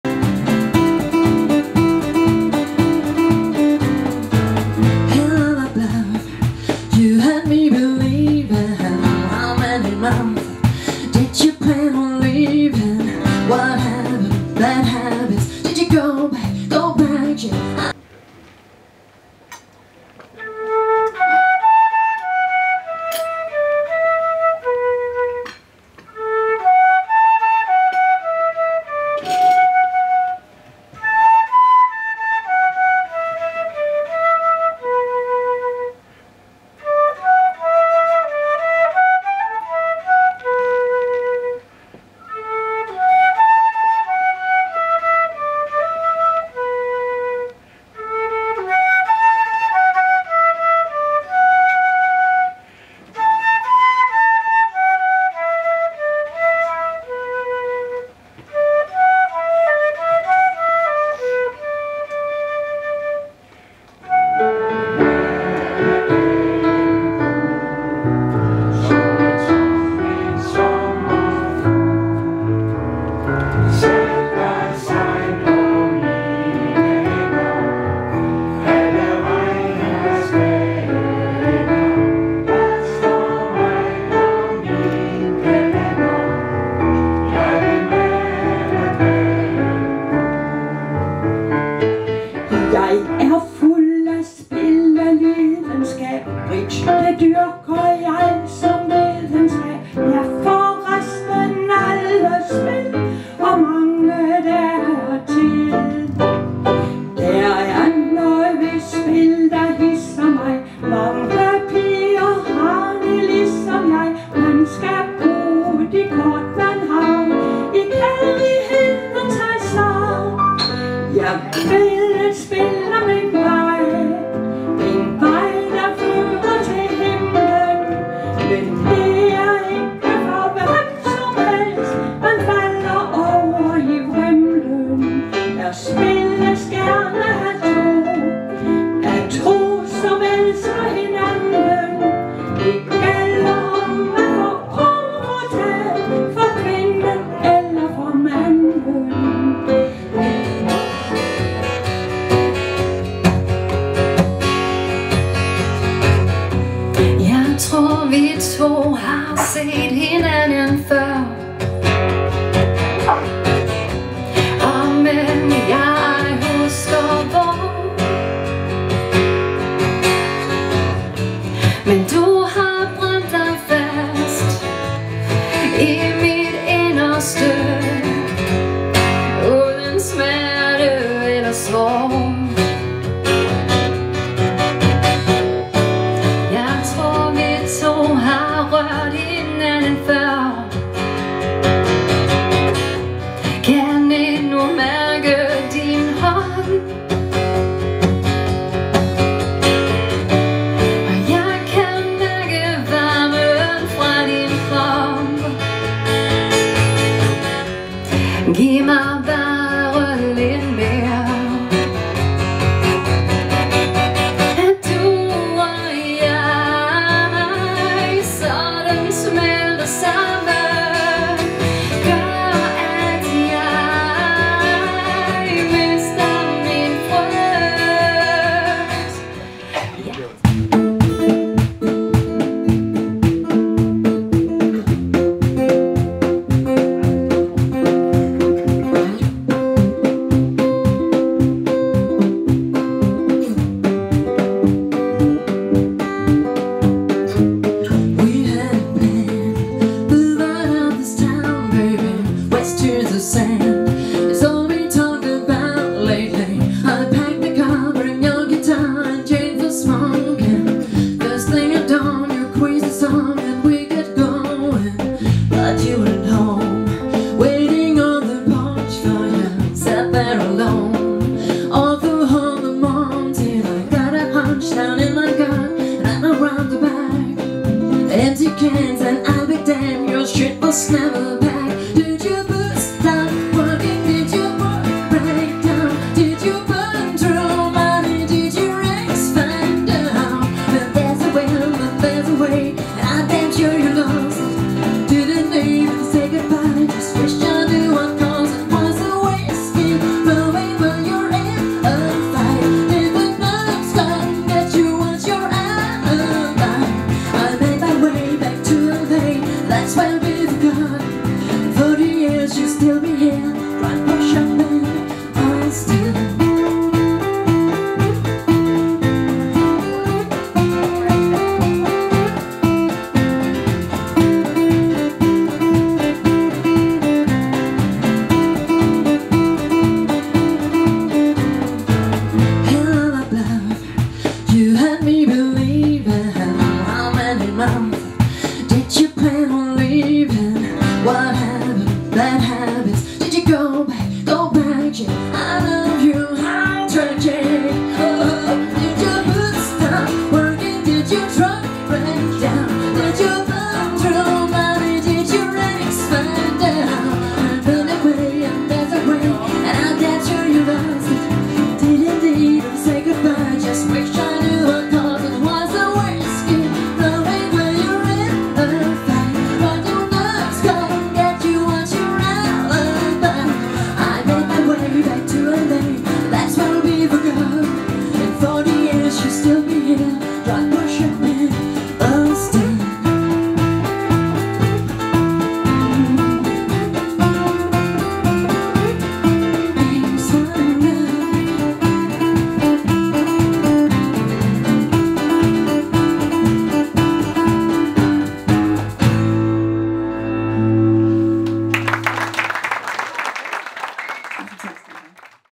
MusikStationen 1 November 2024
Medvirkende:
Hans Henrik tværfløjte
Fællessang med Hans Henrik
Gurli, Søren og Torben Revyviser
Thygesen og Maribo guitar og sang
Helle Maiken Band